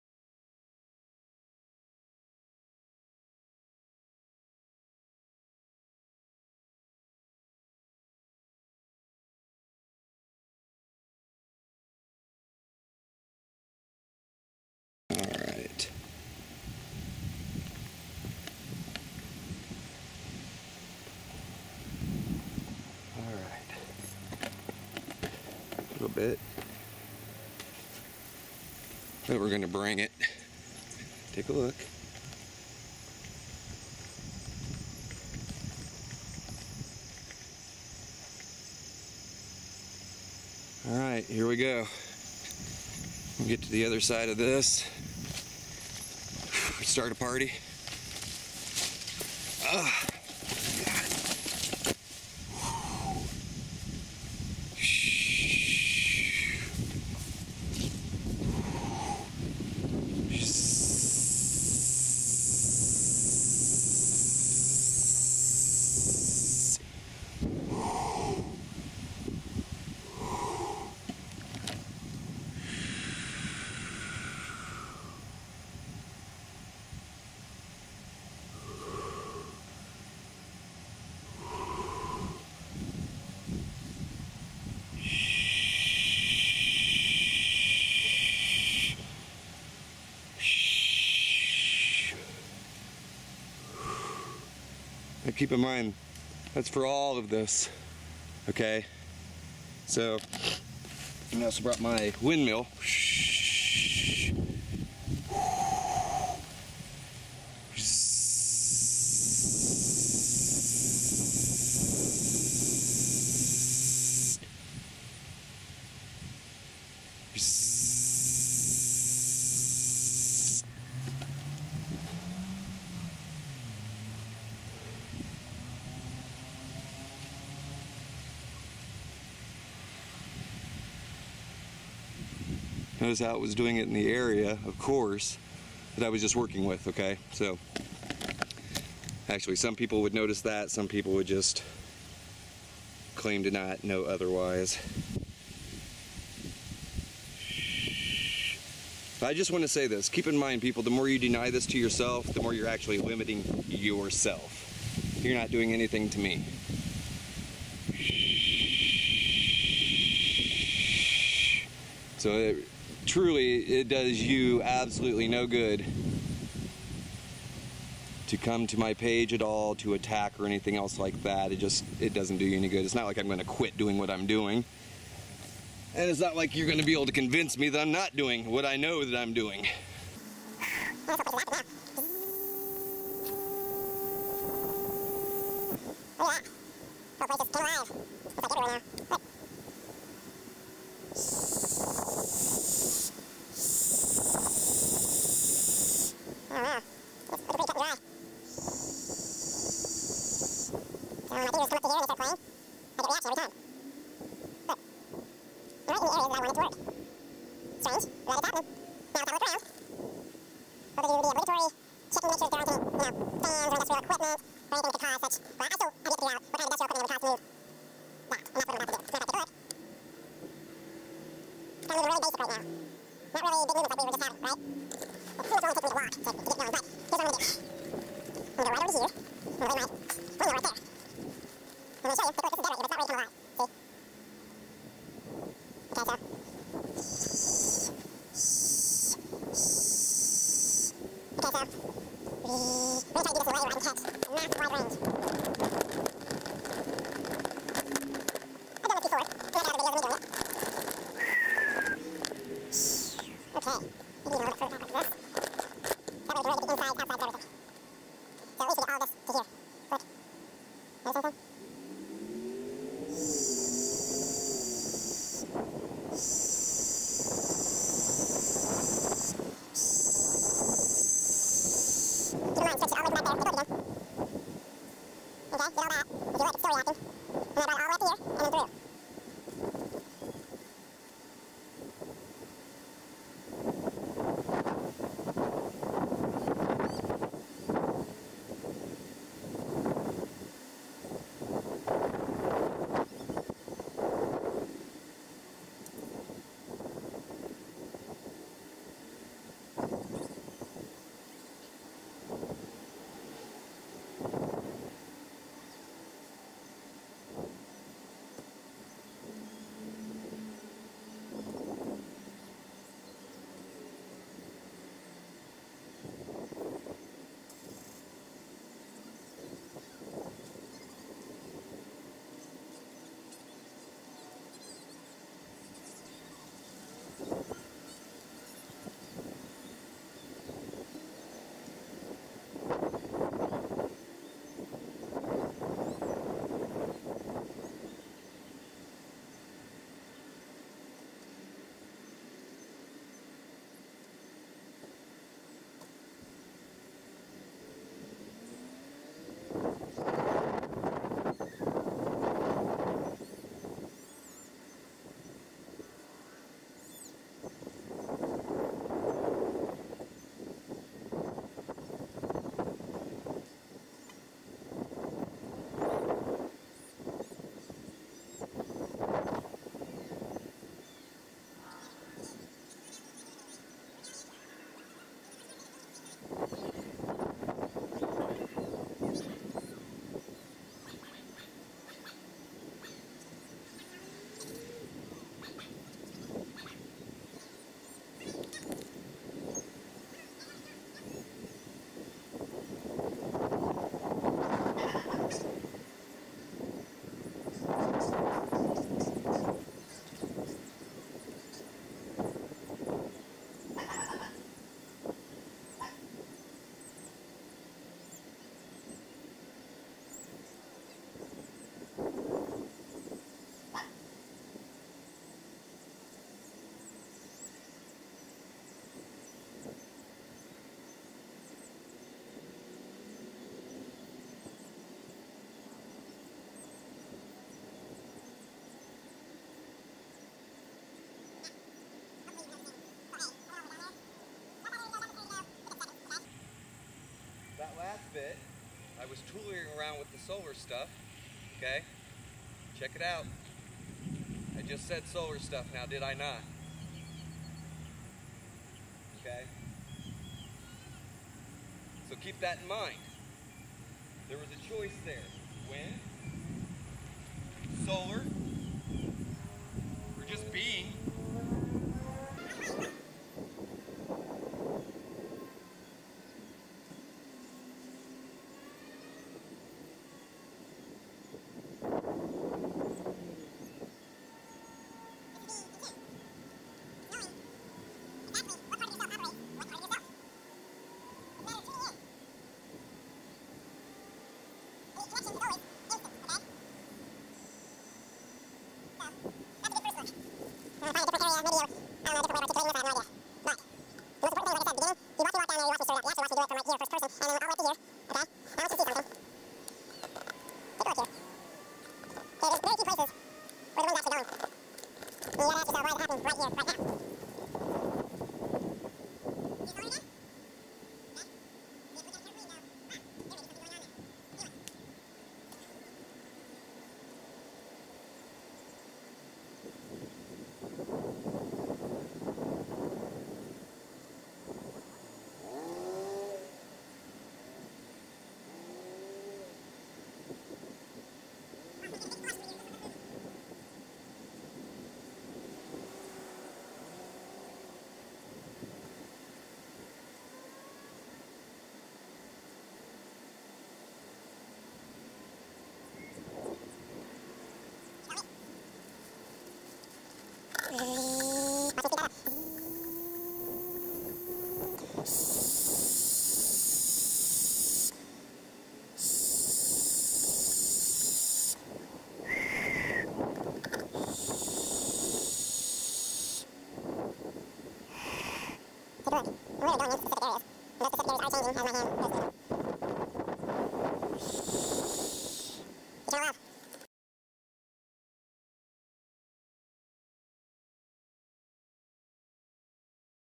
Aerokinesis (Mass Systems)
This video shows the movement of large systems, which of course, destroys any idea that there are outside methods for moving the energy.